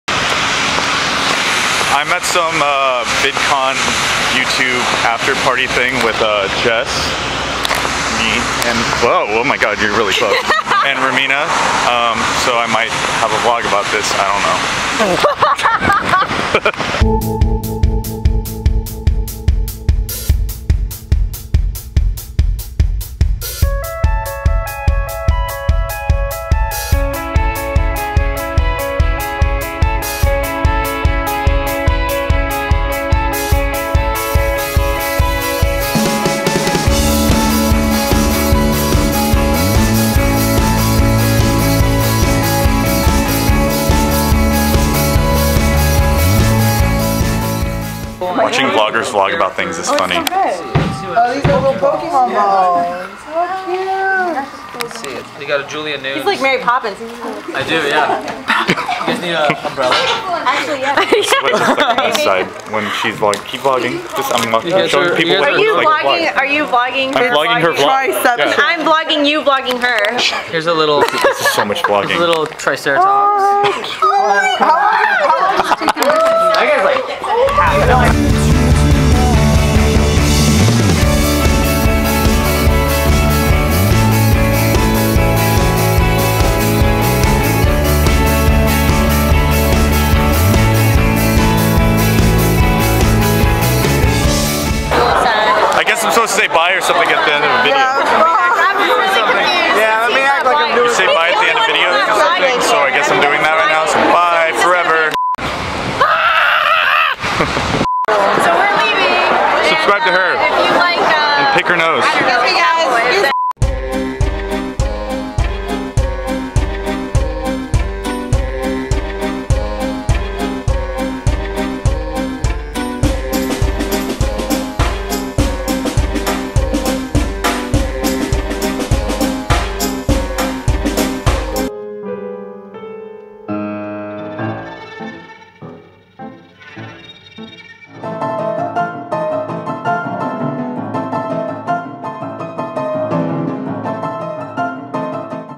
VidCon After Party with redRomina and Everyday Science - Denis Caron Vlogs
I’m vlogging you vlogging her vlogging...
▼☠ MORE INFO & Stalk me on the Internets ☠▼